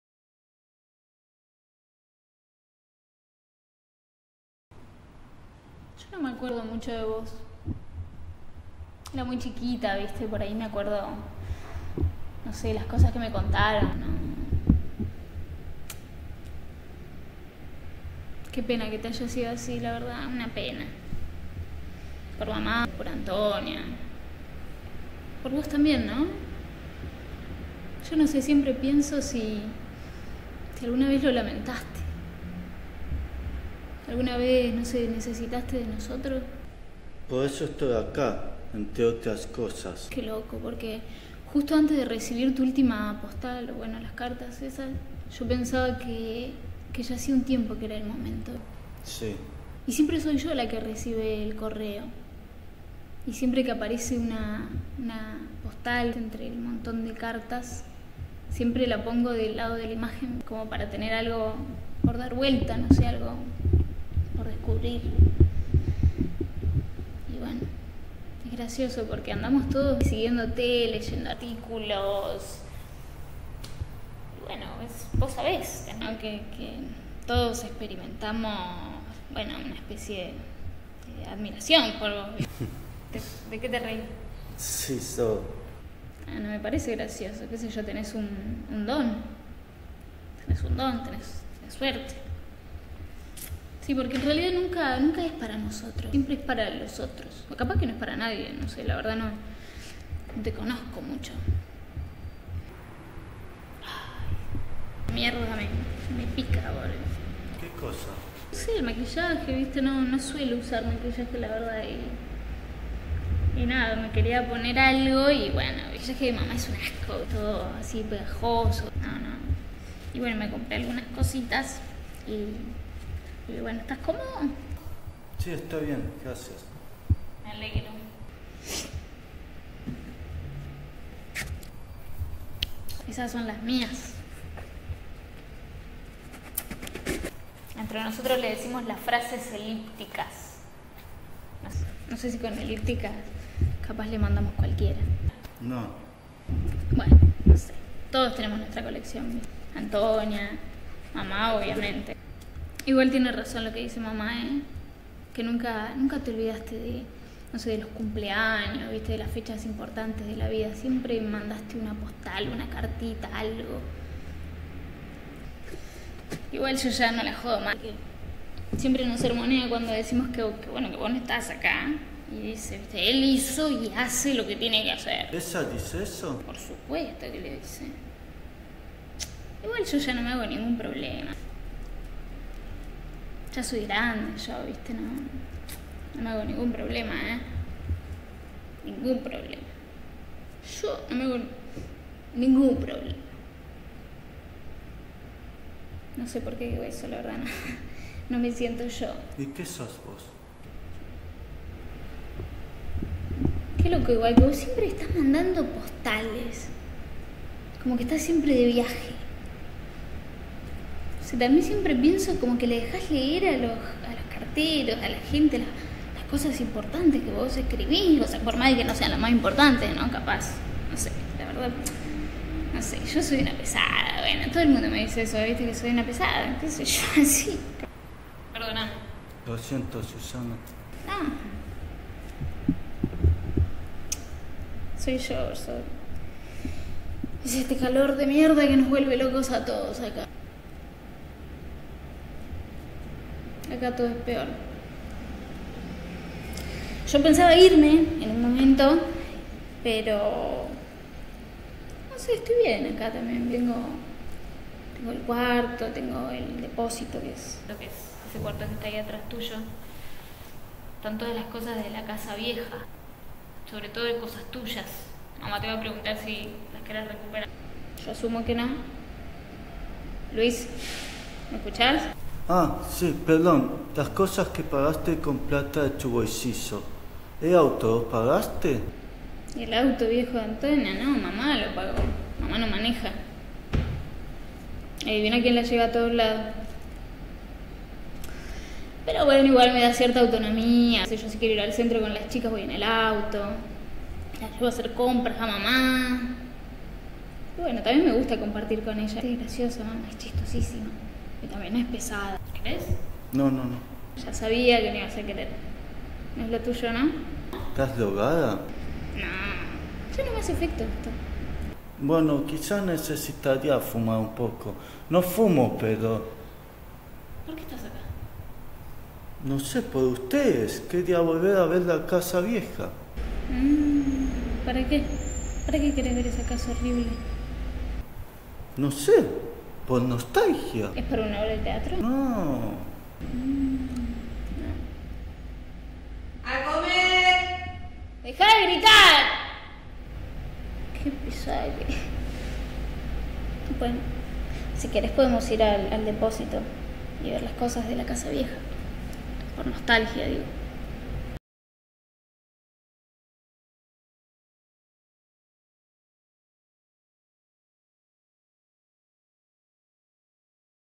Hermanos - Escena 1
Video especial

Seminario intensivo de actuación frente a camara
En rodaje: 2 jornadas filmando escenas

Laboratorio de actuación
La Fusión - Av. Medrano 637 - CABA

Dirección: Sergio Albornoz
Asistencia: Pía Fonseca

Actúan:
Aluminé Guerra - Susana
Leonardo Caparrós - Luis